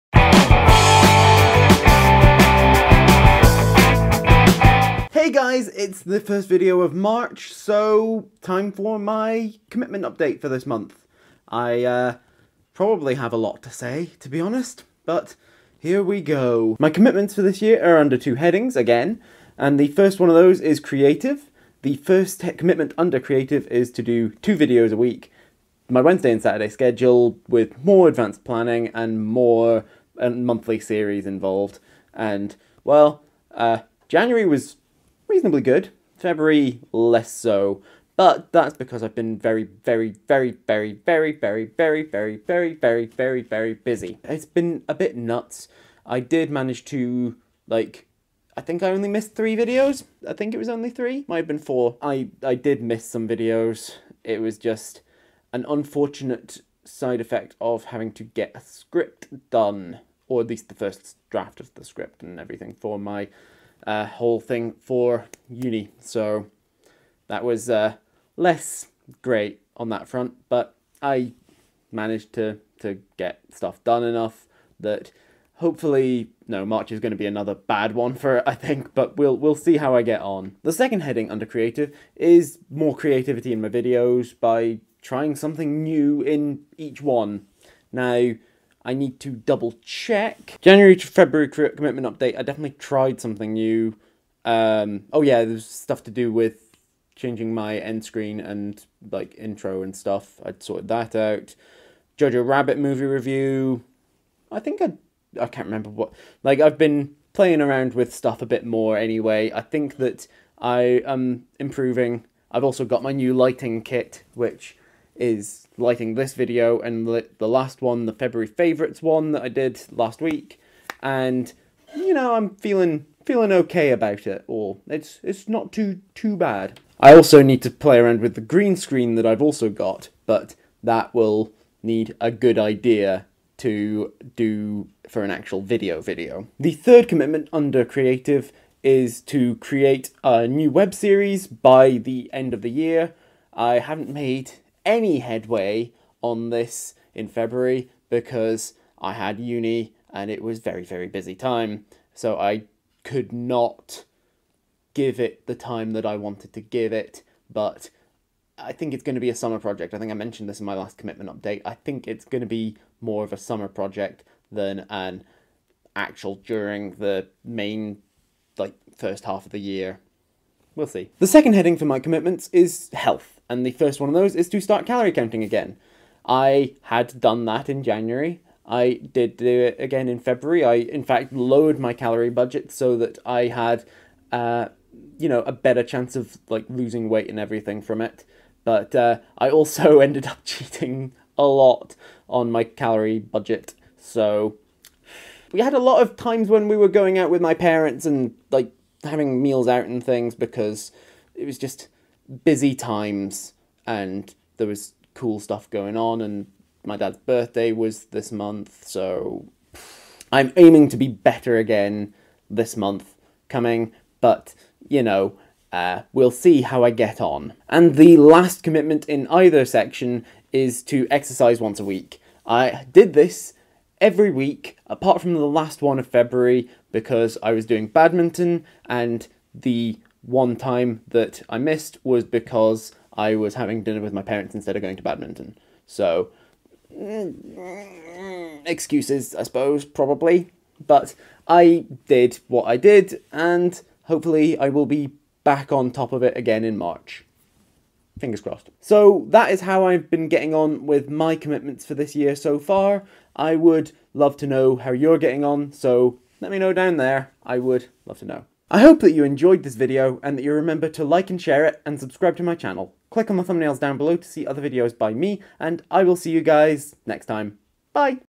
February to March Commitment Update
An update on my 2020 Commitments for February going into March!